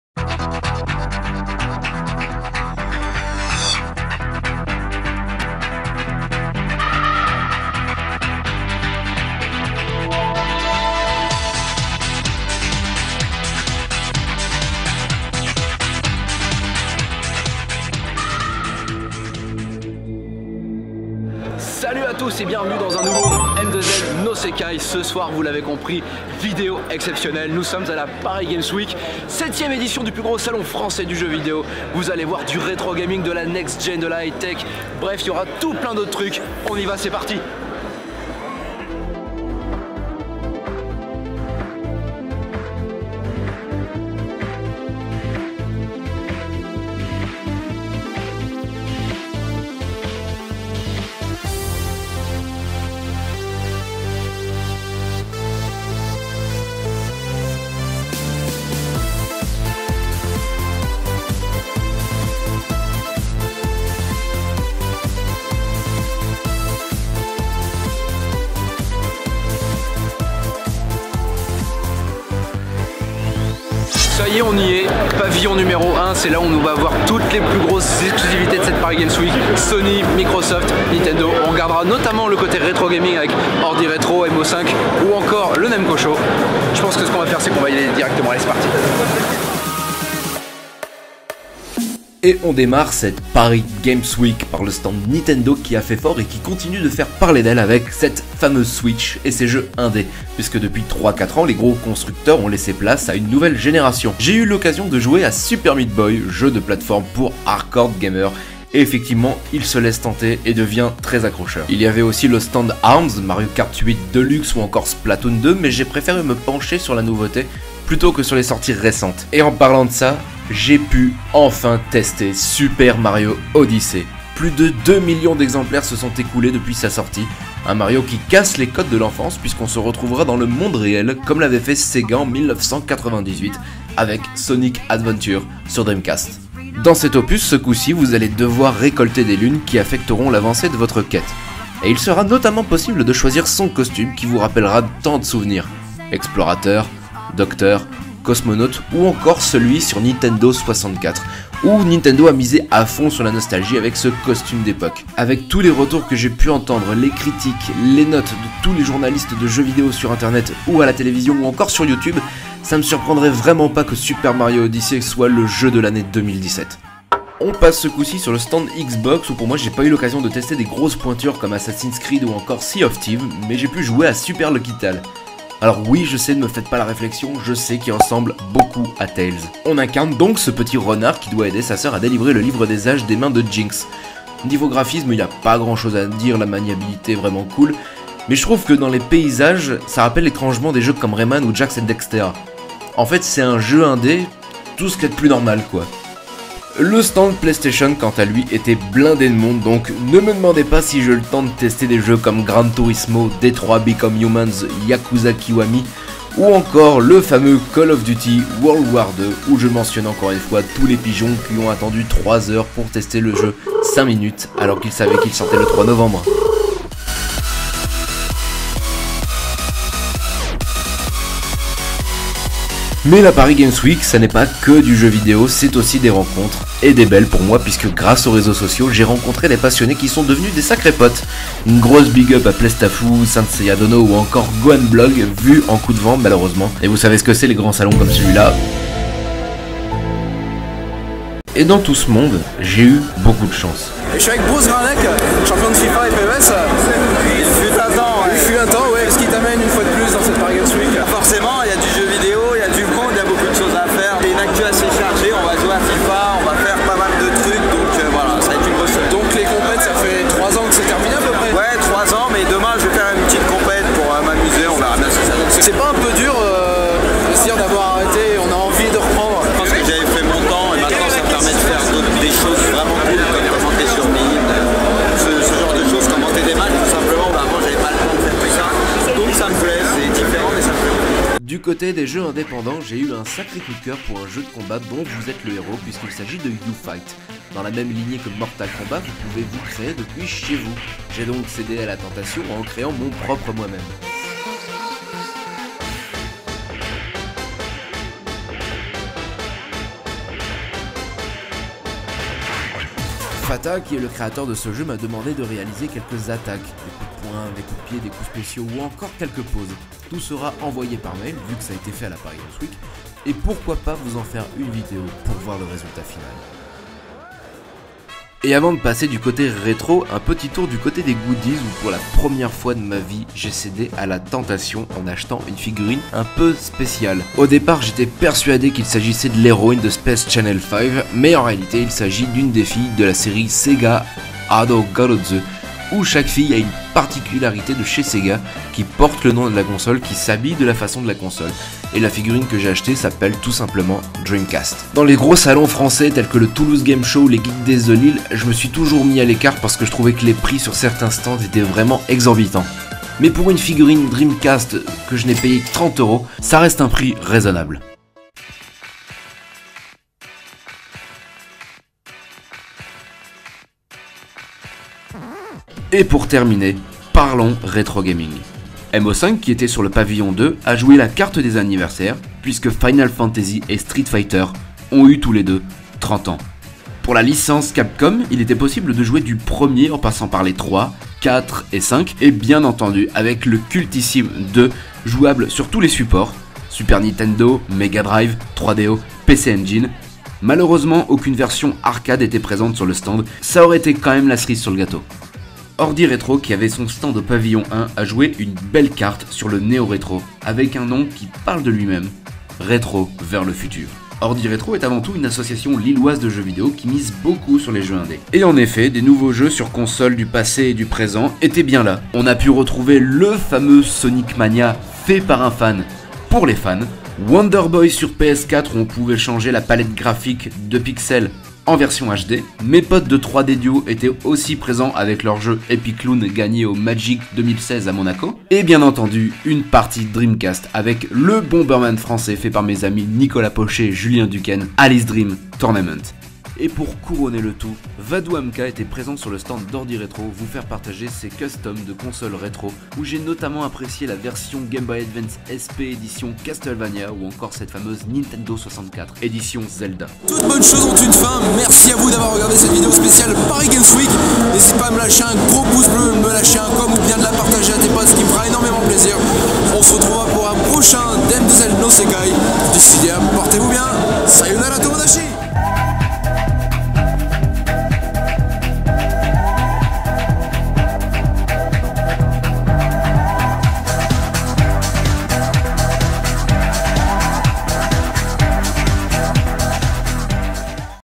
M2L No Sekai - PARIS GAMES WEEK 2017
On signe un 4ème épisode en extérieur ou on vas découvrir les nouveautés gaming pour 2018 à la Paris Games Week
Mais aussi l'occasion de voir des copains et du retrogaming

● INFORMATION ●

● ME SUIVRE ●